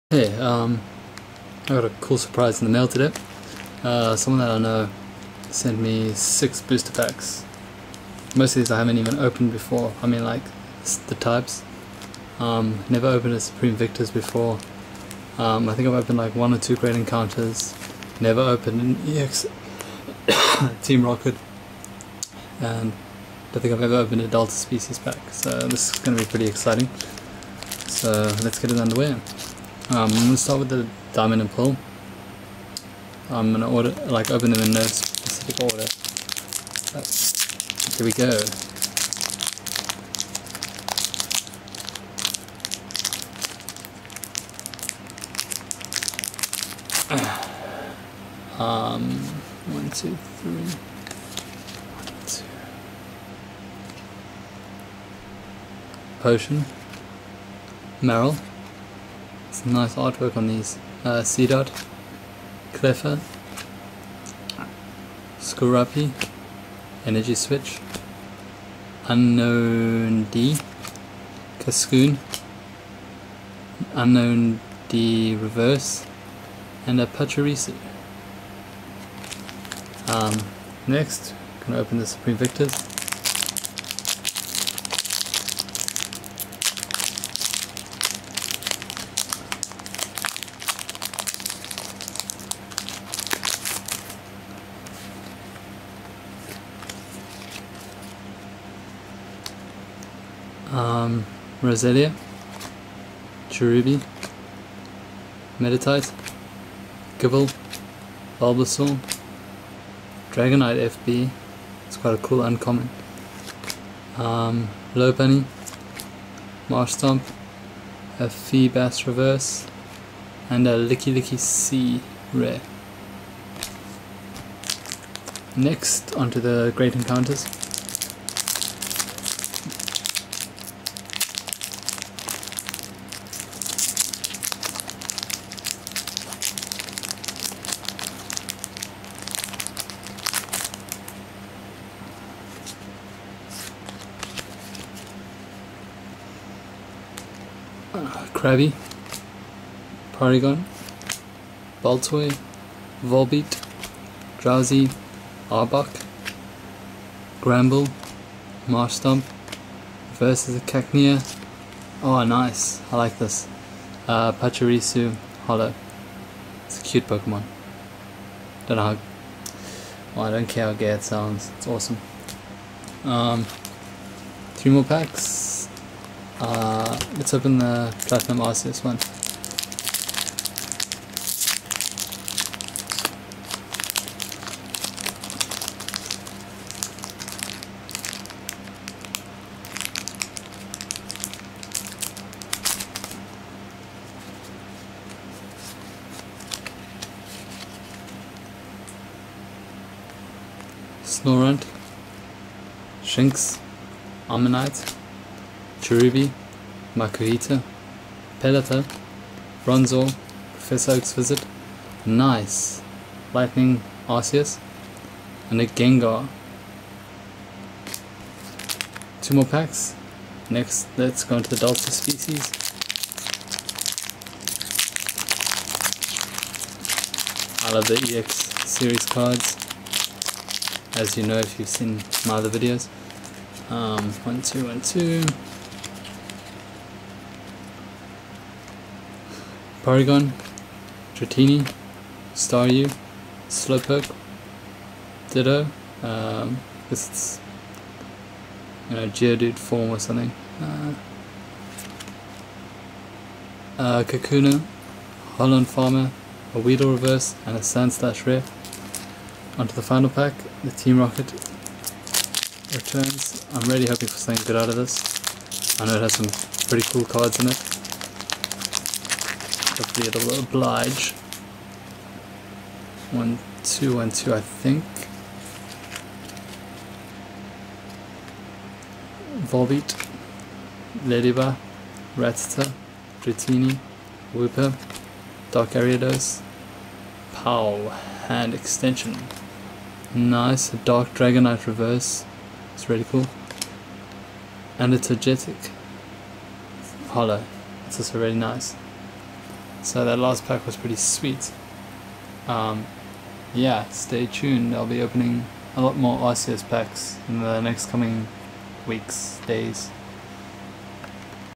Opening 6 Various Pokemon Booster Packs (Good Pulls)
Opening 6 pokemon boosterpacks, Great Encounters, Team Rocket Returns, Delta Species, Supreme Victors and more.